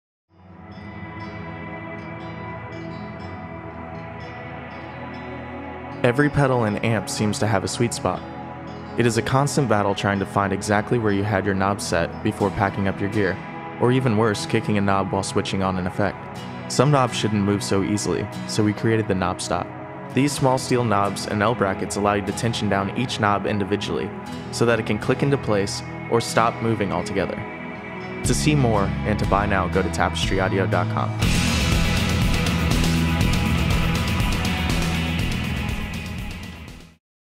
Knob Stop | Tapestry Audio
The Knob Stop is precision machined out of a solid piece of steel and is designed to keep your settings in the right spot. As you tension down the ball plunger set screw with your fingers, the Knob Stop can be locked down completely or can be tensioned so that the knob clicks firmly into place. If you are tired of your settings getting kicked and moved, you should try out the Knob Stop.

*Knob Stop's L-bracket is .30" in diameter which fits mosts standard pots. Please measure before ordering.